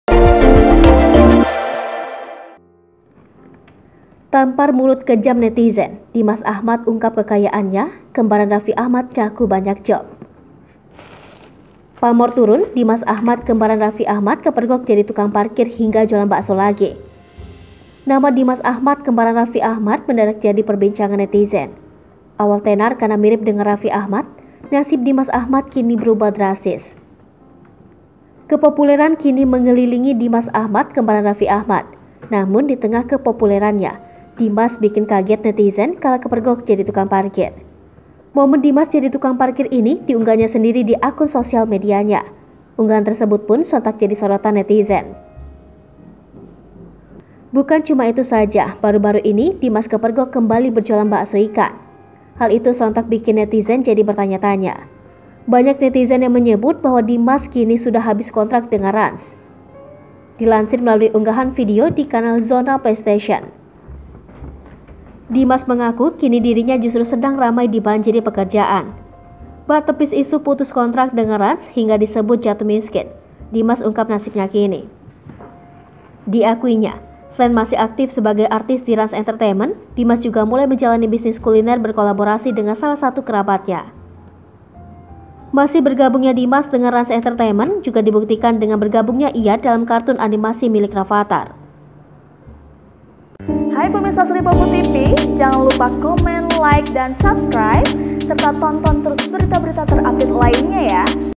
Tampar Mulut Kejam Netizen, Dimas Ahmad Ungkap Kekayaannya, Kembaran Raffi Ahmad Ngaku Banyak Job
Pamor turun, Dimas Ahmad kembaran Raffi Ahmad kepergok jadi tukang parkir hingga jualan bakso lagi, ada apa?

Nama Dimas Ahmad kembaran Raffi Ahmad mendadak jadi perbincanga netizen.

Lihat informasi terkini bersama kami, di:

Palembang.tribunnews.com